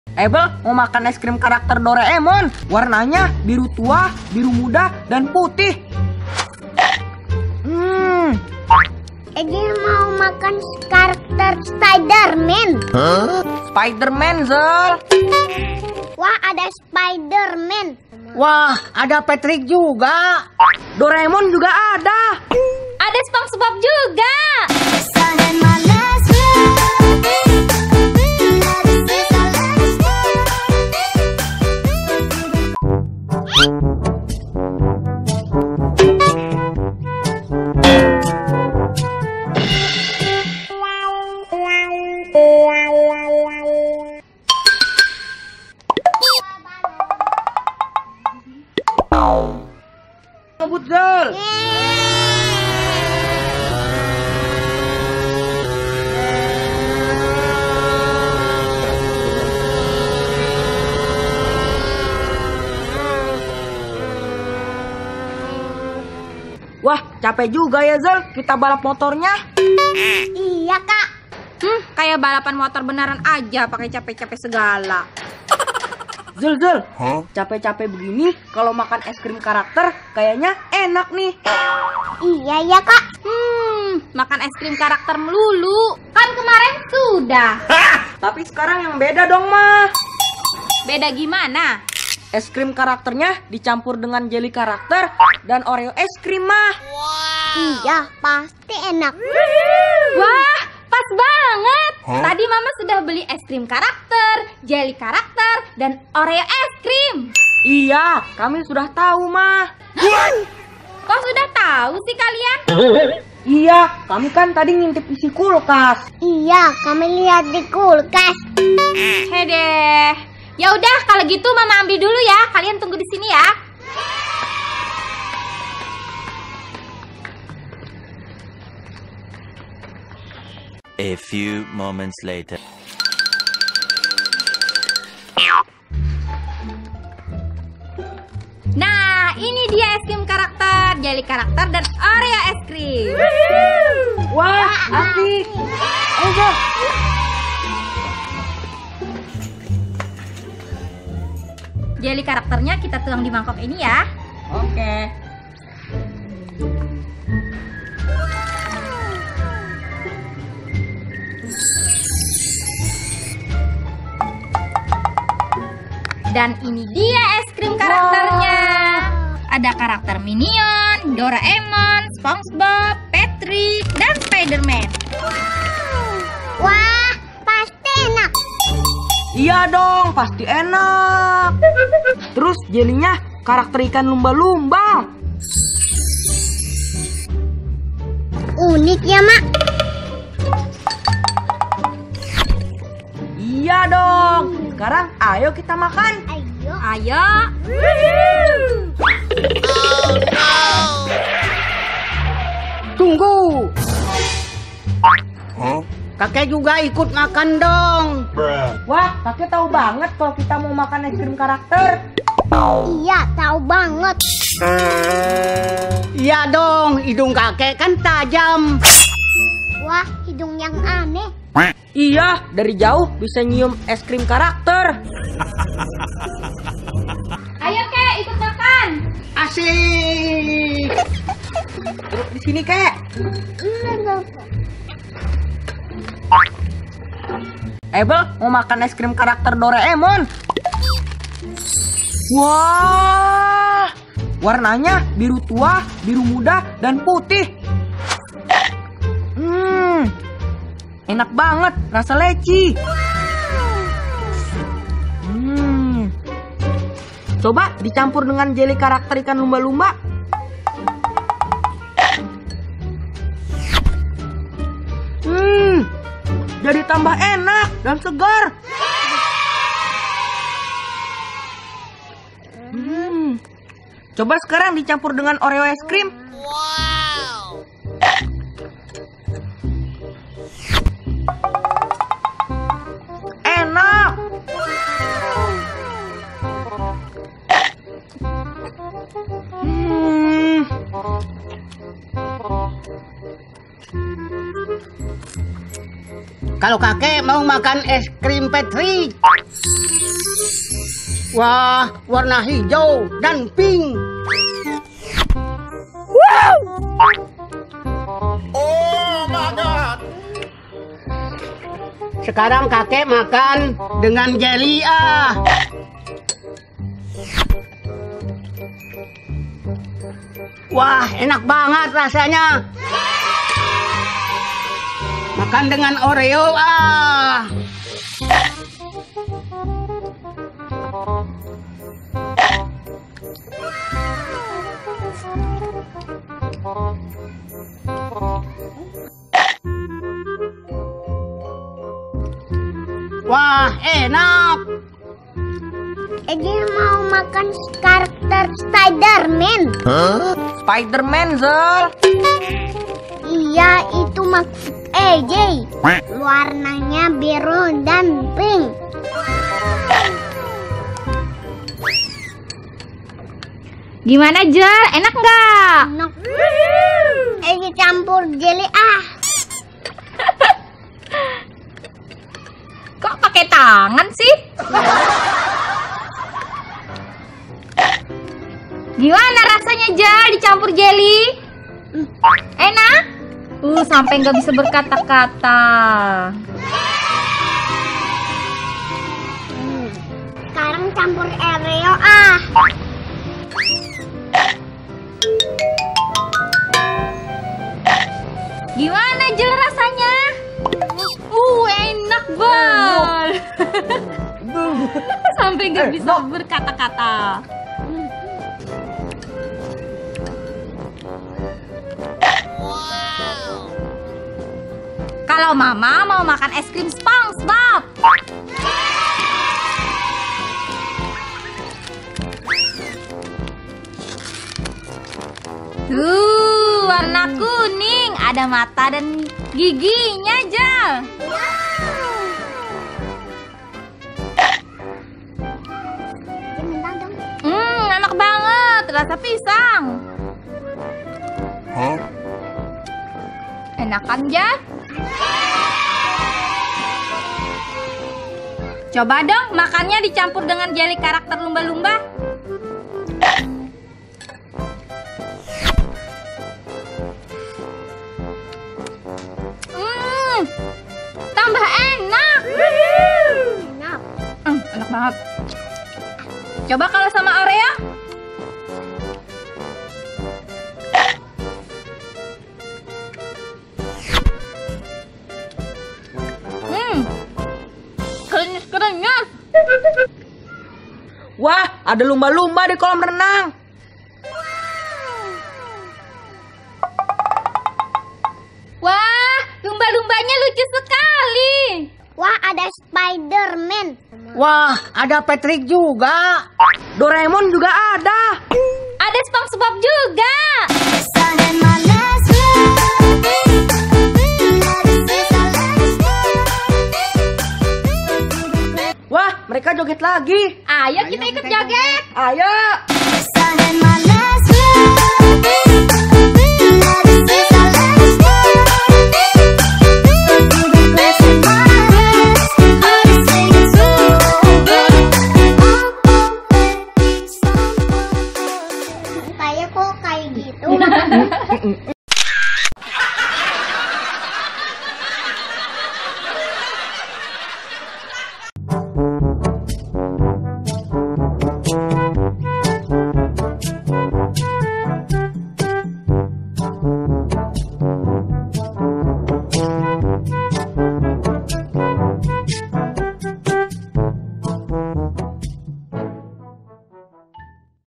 DRAMA MAKAN ES KRIM KARAKTER CAMPUR JELLY DAN OREO | eybel channel
Drama enak gak ya kalau makan es krim karakter di campur dengan jelly karakter lumba-lumba dan oreo ice cream.